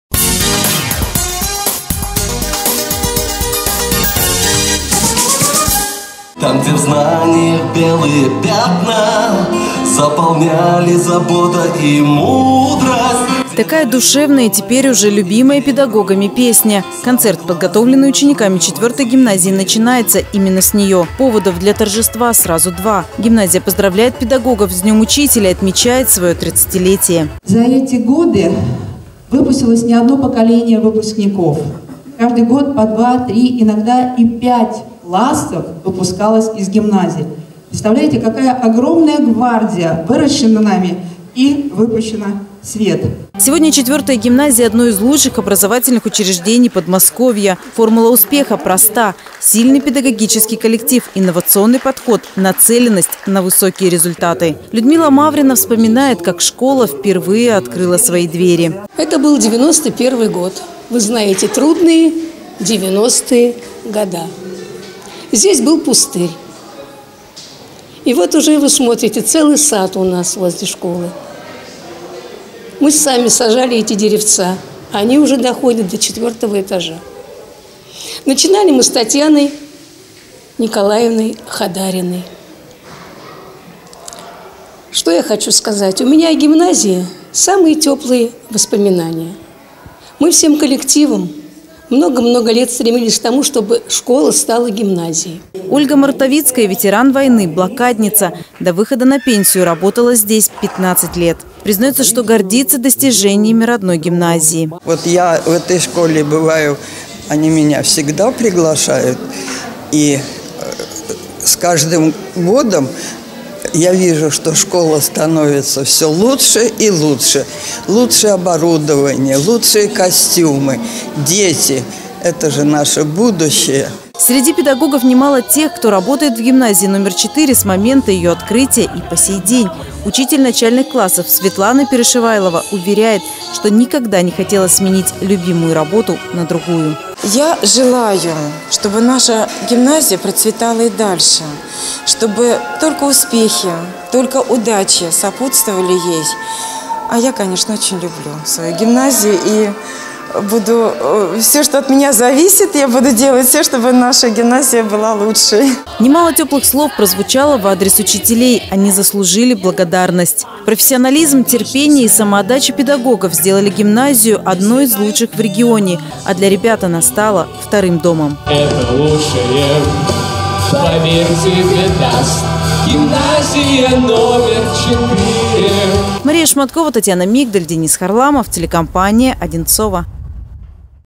30 лет гимназии №4
Одинцовская Гимназия №4 отметила своё тридцатилетие. Торжество приурочили ко Дню учителя.